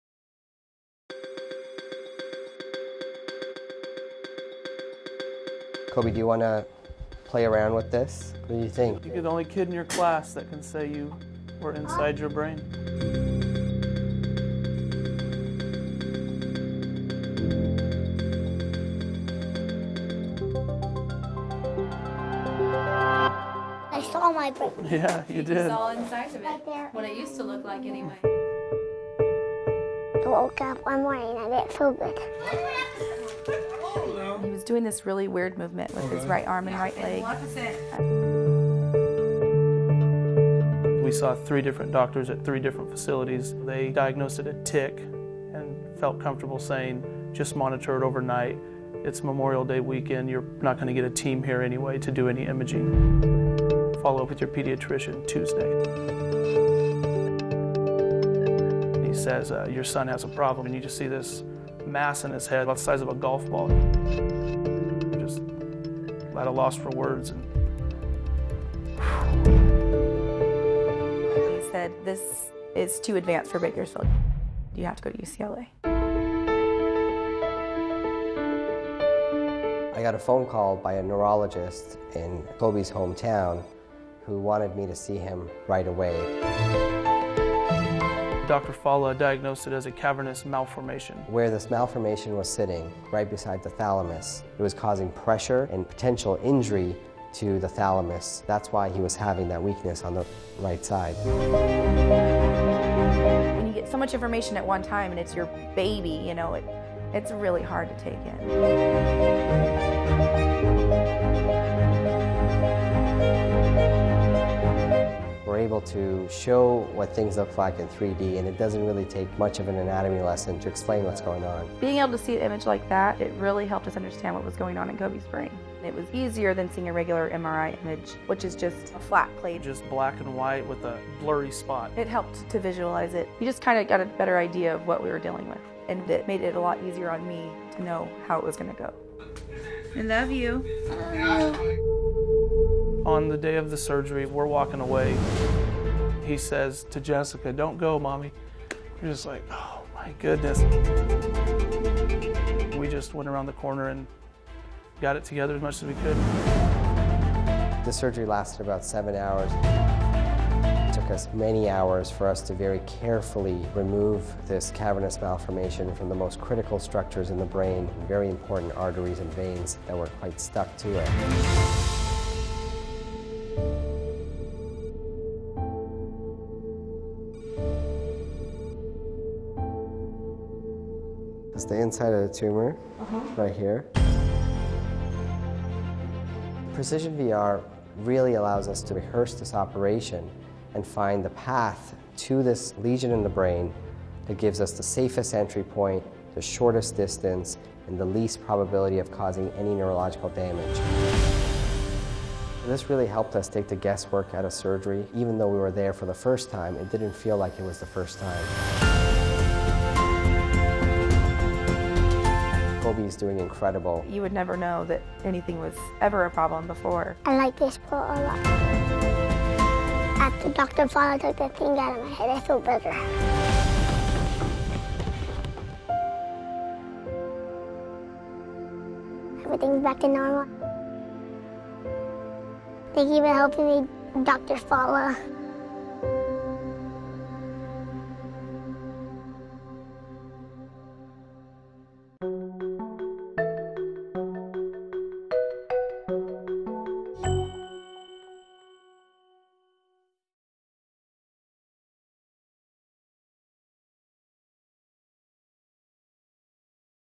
Brain Surgery to the Playground - Kobe's VR Journey
"Brain Surgery to the Playground"

Take a VR Journey with 7-year old Kobe Morrow and his family. They faced a difficult diagnosis when they learned their youngest family member had a cavernous malformation in the brain. Pediatric neurosurgeon, Dr. Aria Fallah and Precision Virtual Reality™ helped them understand how this tangle of abnormal blood vessels in Kobe's brain was affecting Kobe's motor abilities and how surgery could help. See how Kobe is doing now in this Surgical Theater video.